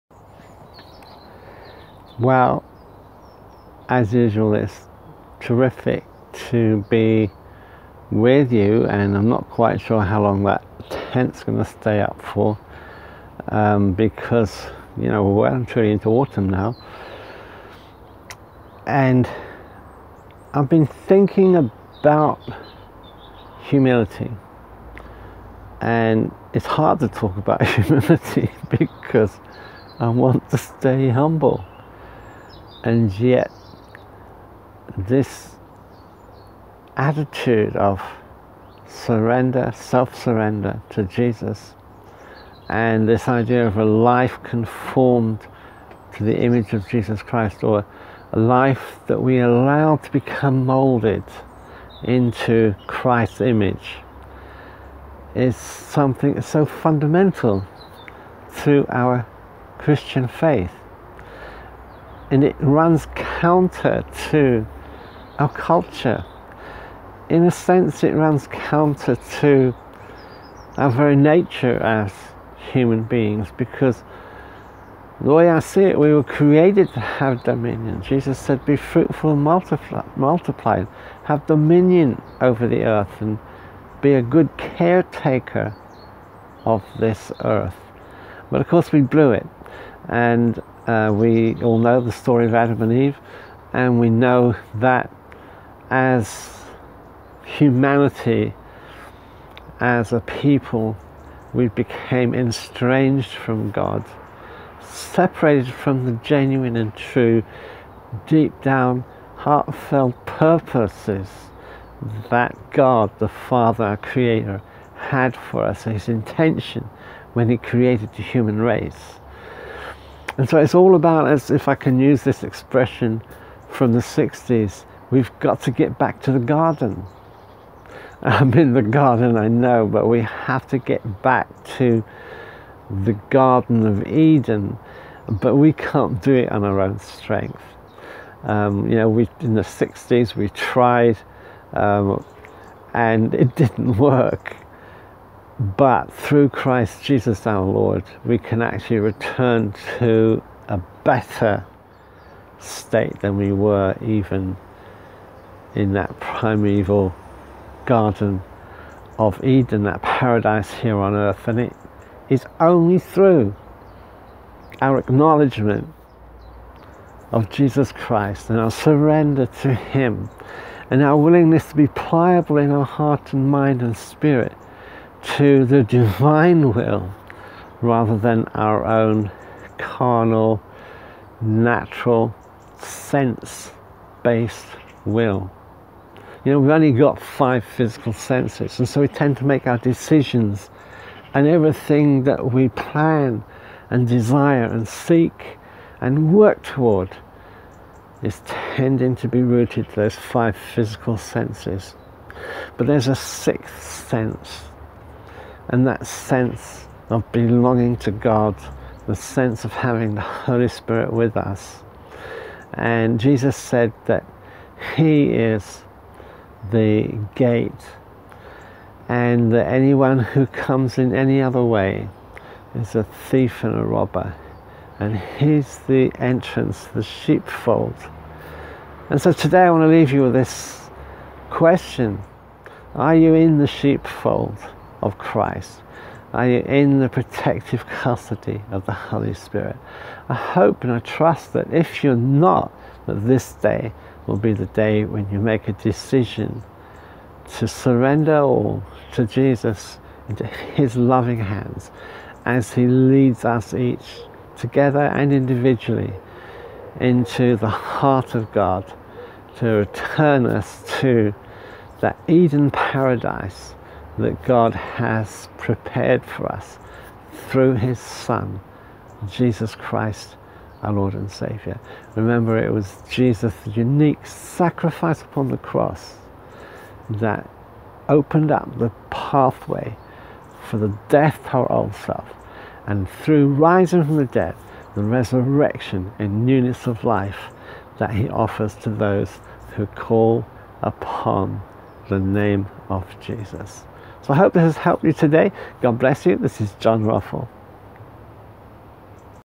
1/7 "HUMILITY: AN INTRODUCTION" Heritage Coffee Time Series w/ John E. Ruffle. 5'00"
We look back almost three months to September 2020 for this HERITAGE COFFEE TIME SERIES, featuring talks I gave that you more than likely missed first time around!

In today's recording  I look at the dynamic of humility - what it is, and how we need to seek to walk in it.

- John E. Ruffle, December 3, 2020.
- This HERITAGE SERIES was recorded September 7, 2020.